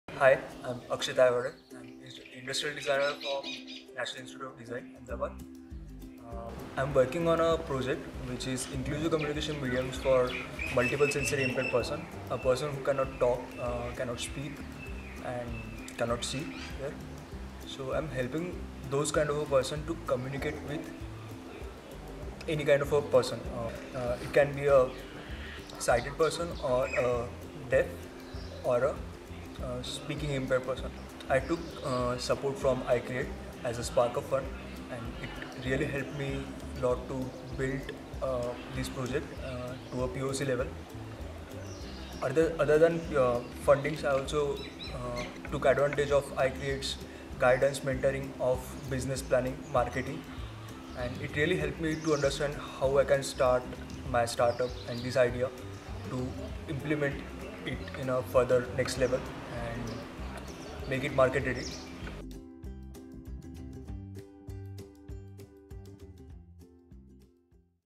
Inclusive Communication Mediums- iCreate Supported Startup
A person with impairment in hearing, speech and sight are known as Deafblind. They often find difficulties in communication which results in their unstable lifestyle. Many efforts have been made in the direction of innovating a support system for them.
'Inclusive Communication Mediums' founded by Akshay Taiwade is developing mediums for deafblind people. It enables easy communication by bringing them on a common platform to connect with all set of people.
He has received Sparkup Grant from iCreate to develop the POC.